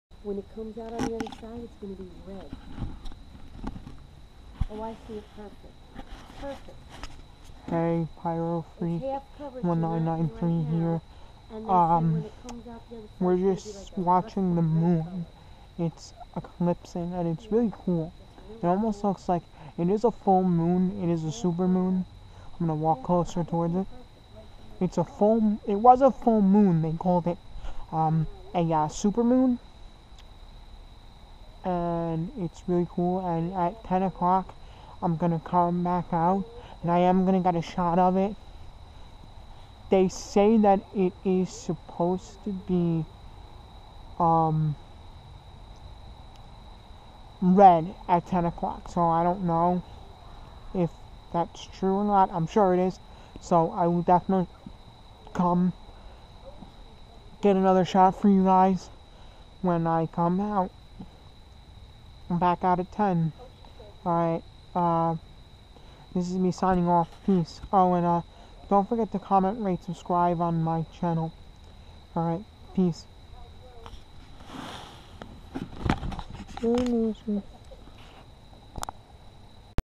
Lunar eclipse part 1
Hey guys this part 1 of part 2 if my lunar eclipse video and just me talking about other vids i have that i am going to upload to my channel soon have some exciting things that are happening in my life with my family